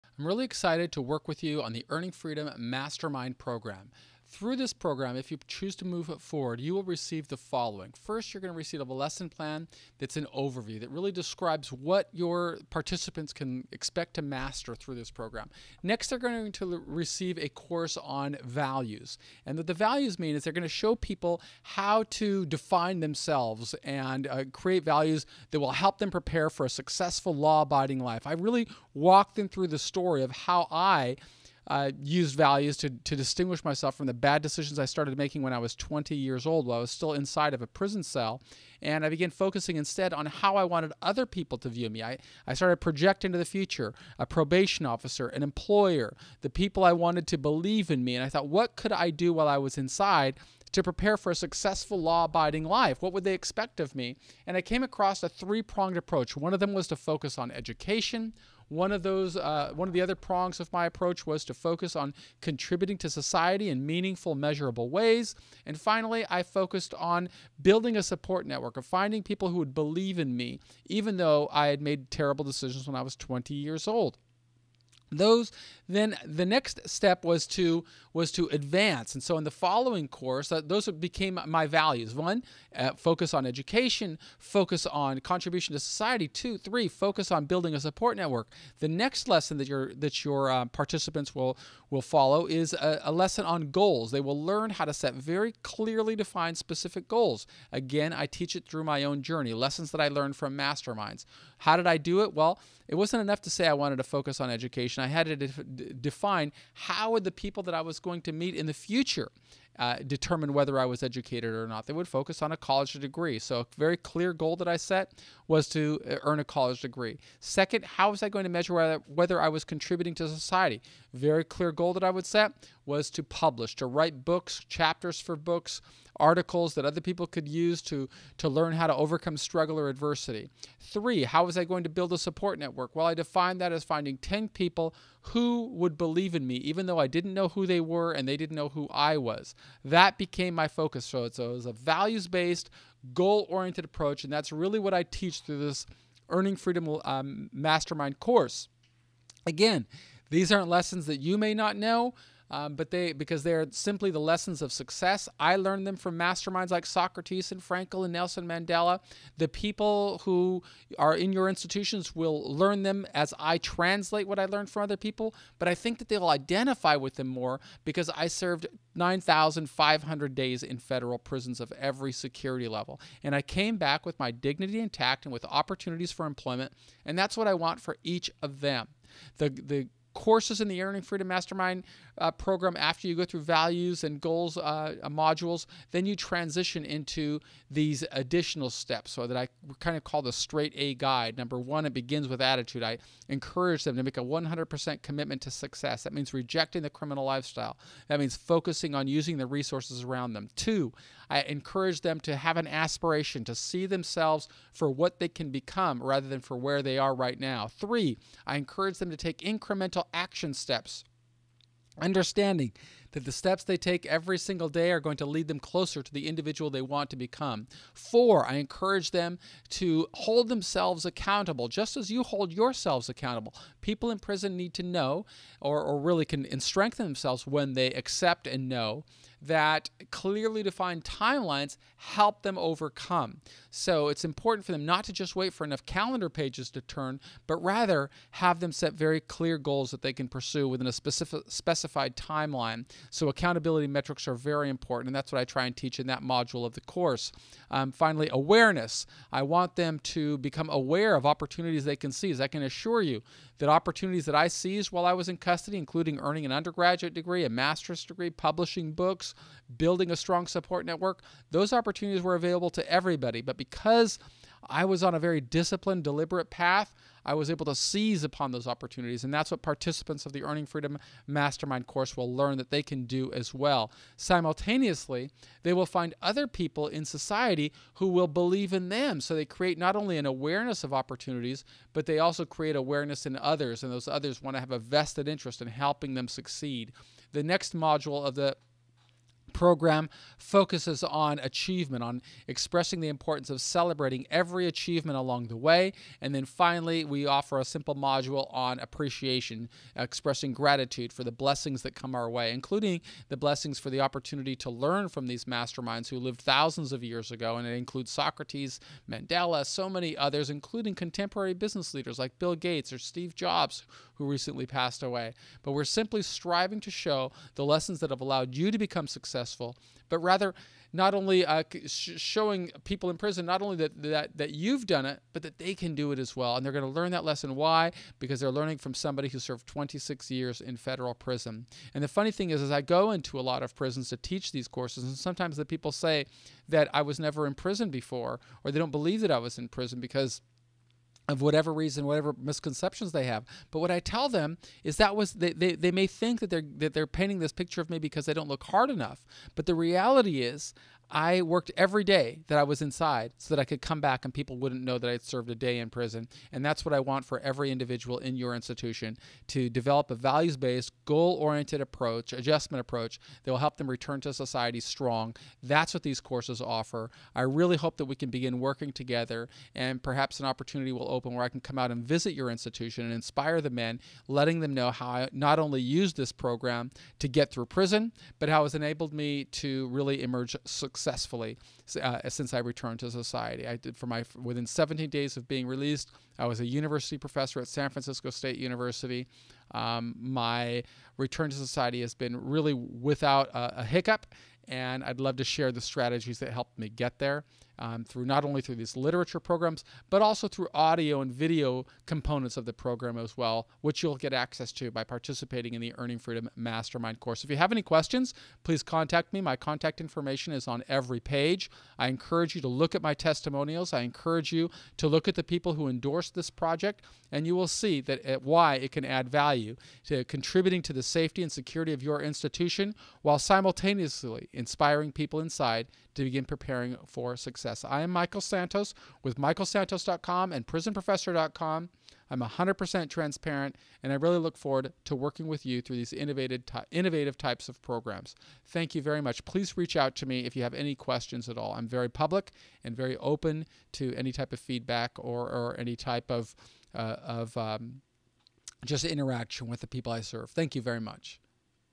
Earning Freedom Sales Page
Prison reentry program offering lessons that empowered me through 26 years in federal prison. Lessons from Socrates, Nelson Mandela, Viktor Frankl, and others who overcame struggle of imprisonment.